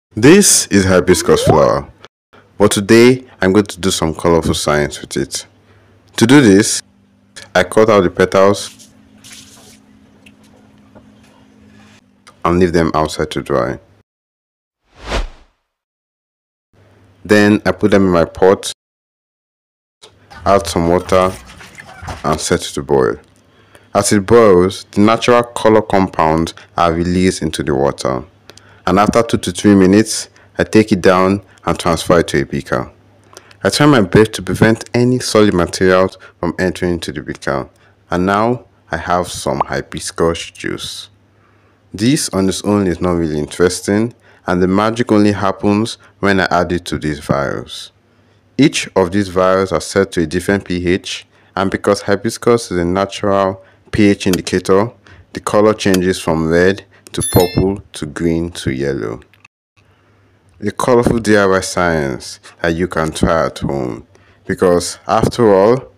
The Chemistry of Hibiscus | Nature's pH indicator
Did you know a simple hibiscus flower/China rose can tell the difference between acids and bases?
In this experiment, I used hibiscus as a natural pH indicator — and the results are a stunning rainbow of colors!

This is one of the coolest DIY science experiments you can try right at home! 🧪

 Watch till the end — so you can try it yourself